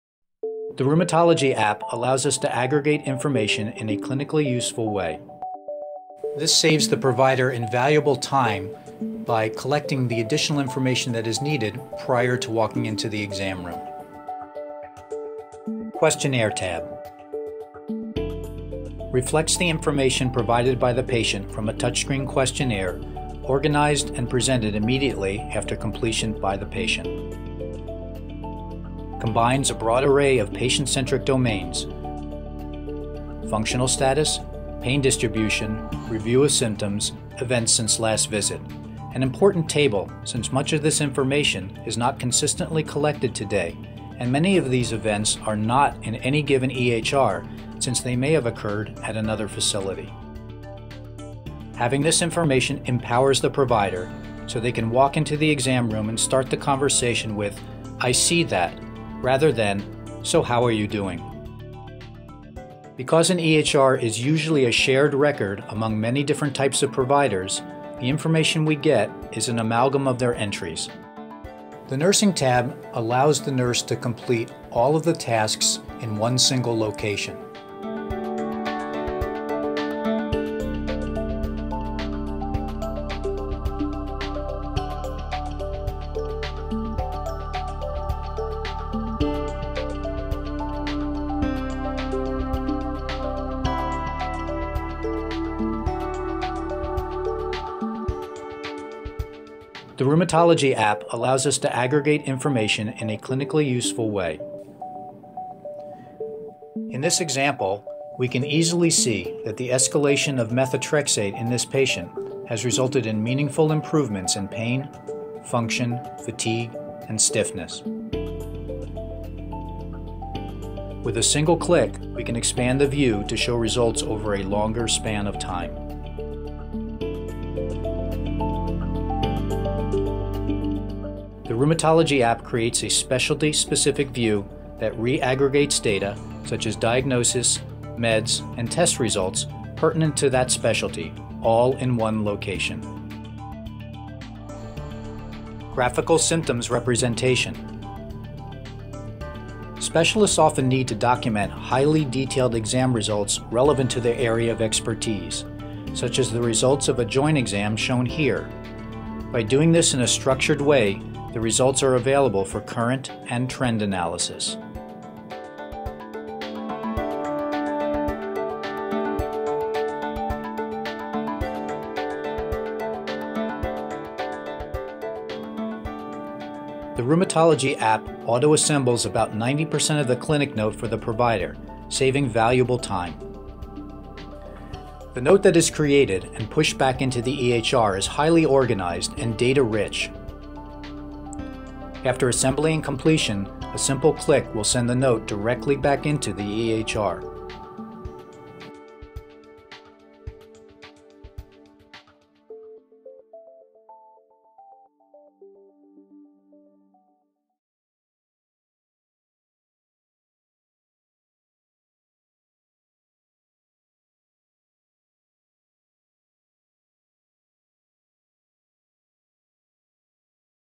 xG Health Solutions: EnrG | Rheum
Improves care for rheumatology patients. Extracts, organizes and presents patient-entered data with EHR data in a user-friendly and clinically-relevant way.